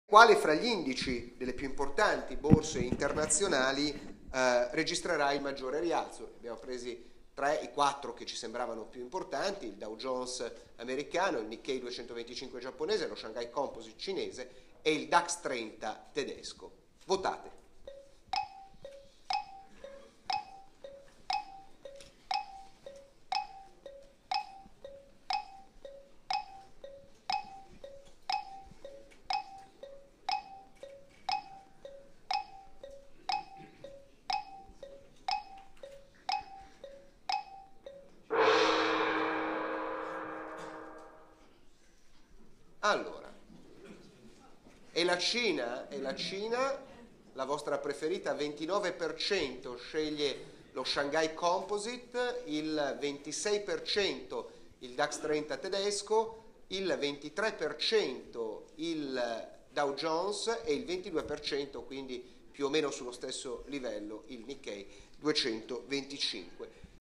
Usa, Cina, Giappone e Germania: ecco dove vincerà il Toro
Bluerating ha raccolto il parere degli operatori che hanno seguito il convegno "Outlook 2015, dove vanno i mercati". Questa la loro opinione